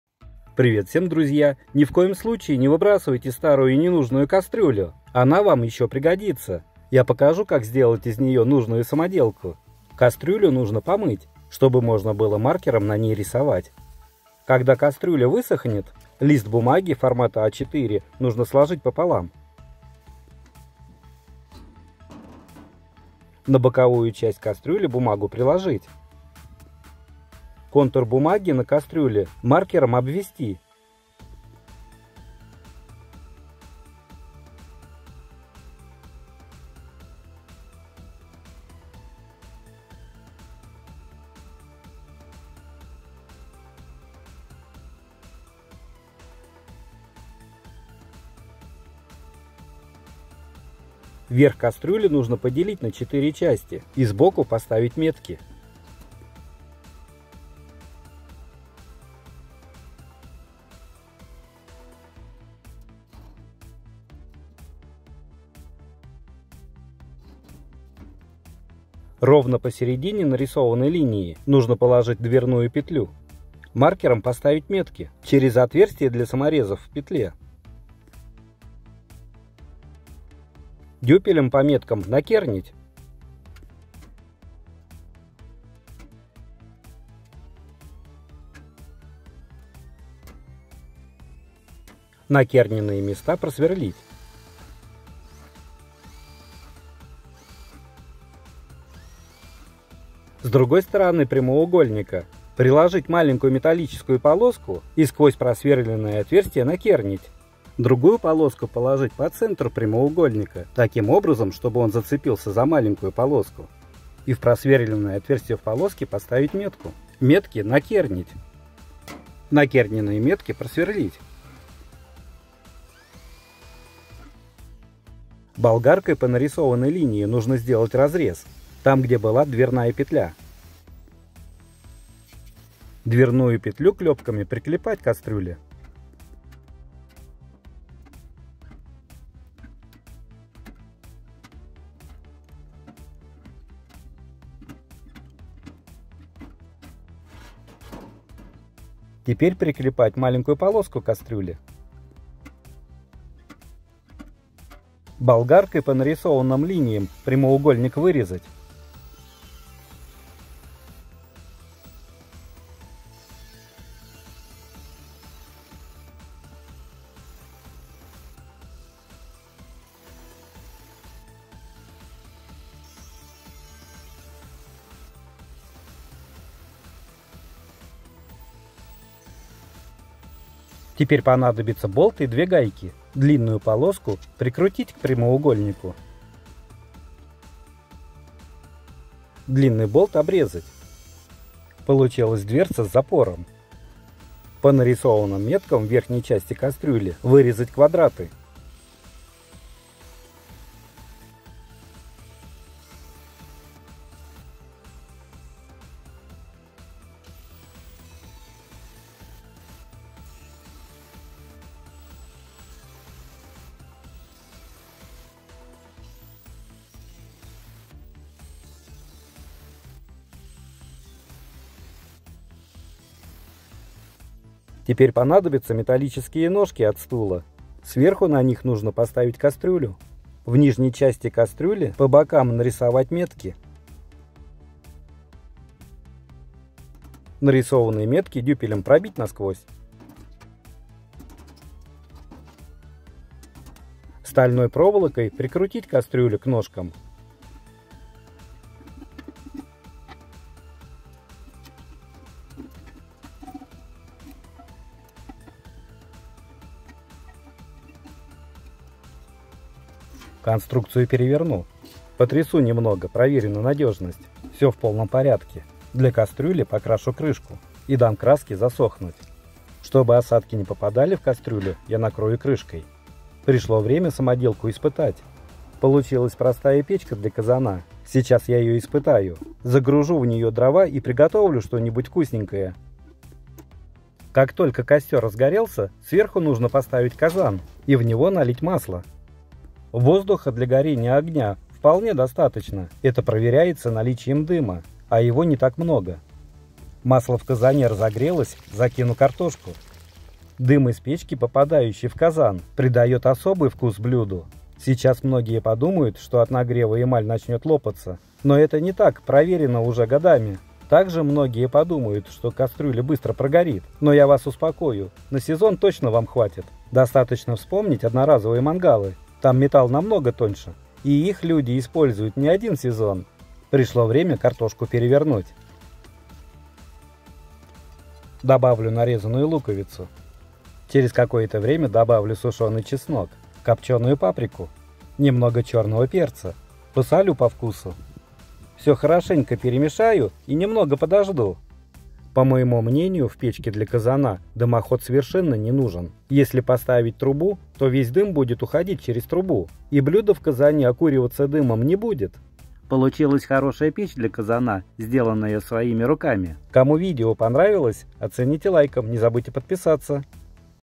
Ни в коем случае не выбрасывайте старую ненужную кастрюлю. Хорошая печь для казана своими руками.
В этом видео Вы увидите как сделать своими руками нужную самоделку из старой ненужной кастрюли. Получилась печь для казана. Поставьте пальцы вверх  Жду Ваше мнение в комментариях.